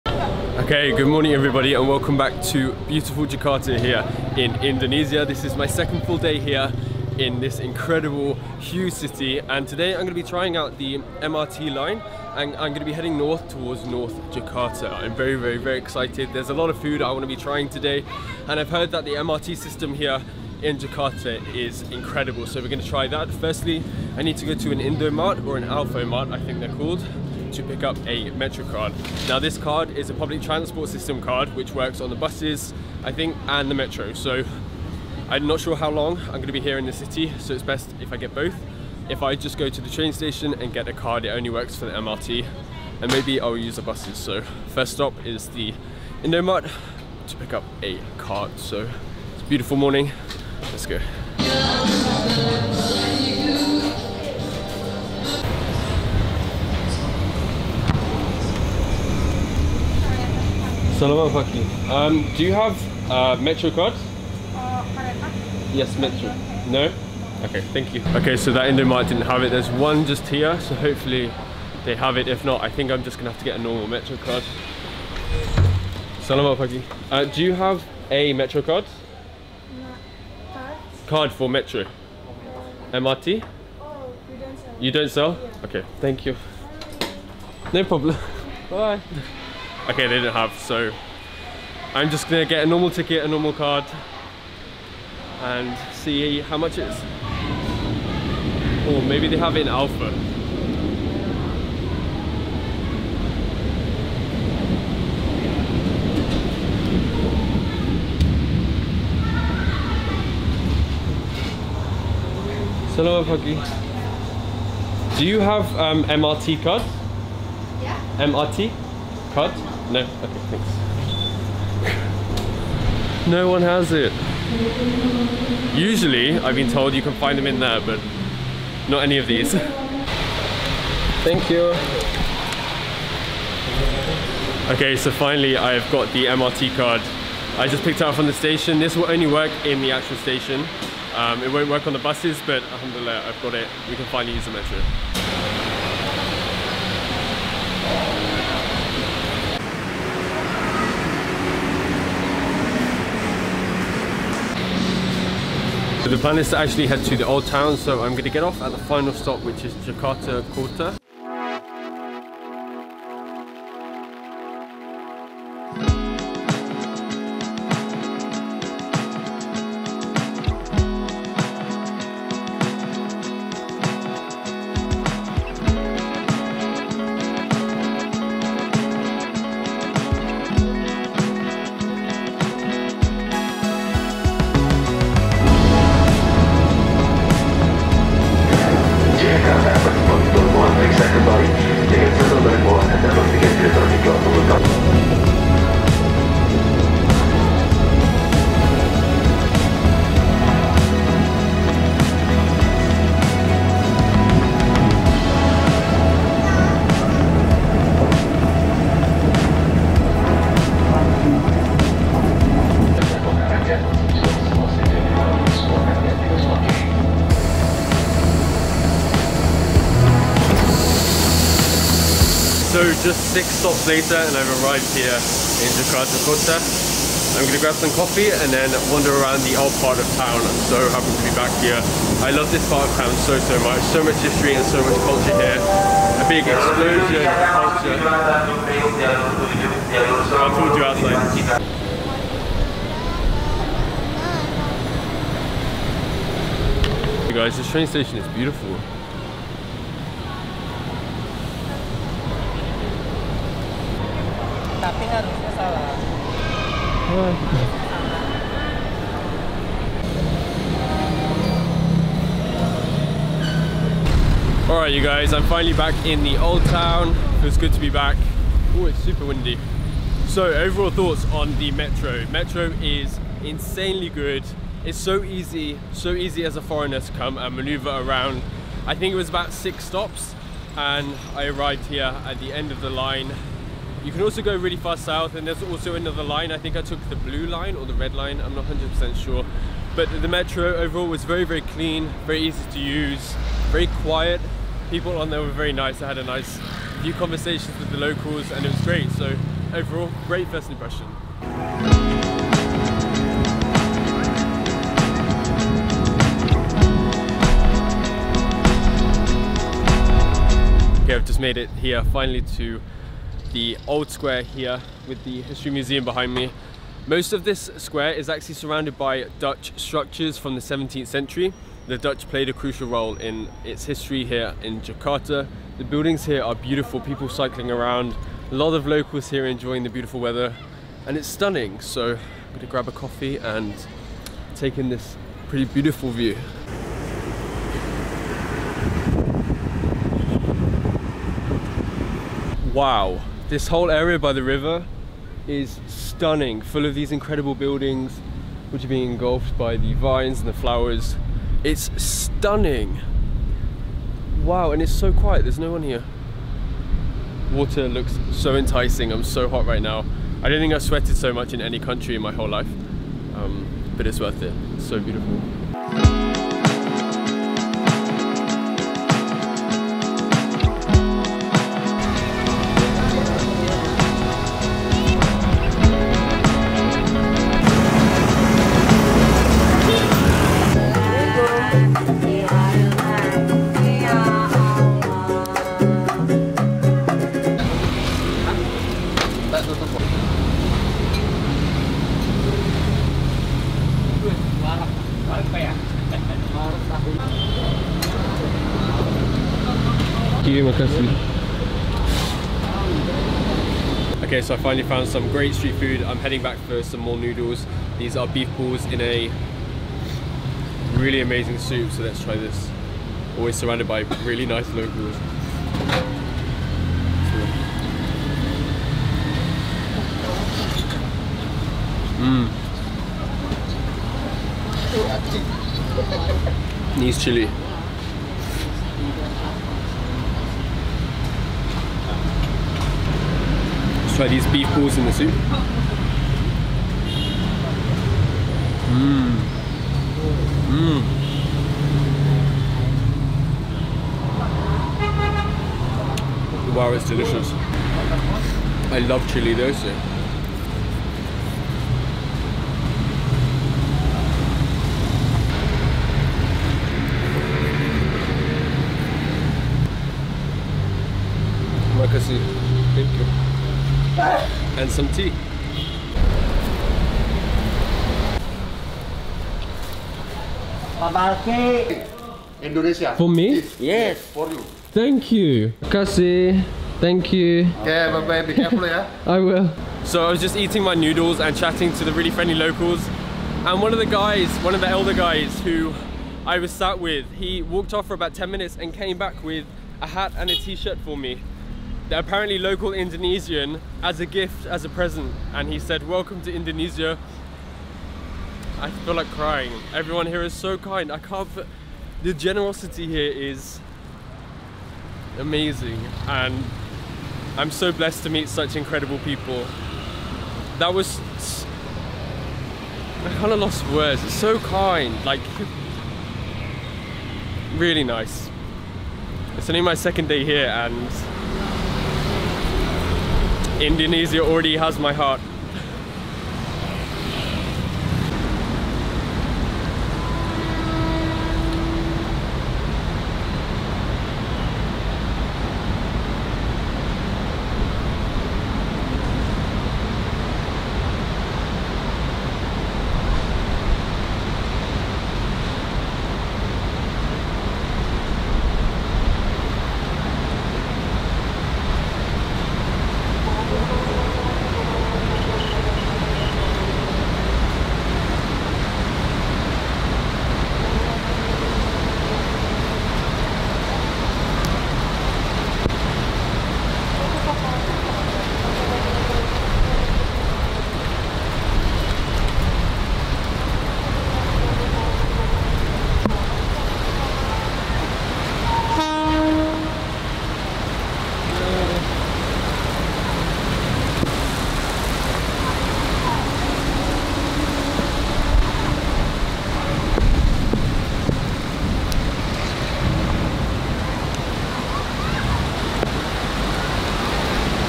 trying JAKARTA'S COMMUTER for the first time | Jakarta Kota Old Town 🇮🇩
trying Jakarta's Impressive MRT and exploring Jakarta Kota (Old Town) 🇮🇩